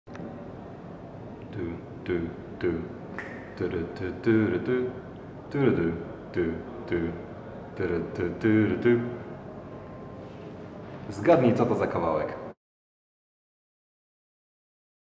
HummingChallenge iSpot!
Odgadujesz wszystko po jednej nutce?

Śledź kanały iSpot i weź udział w HummingChallenge! Sprawdź się i zgarnij karnety na Polish Hip-Hop Festival :)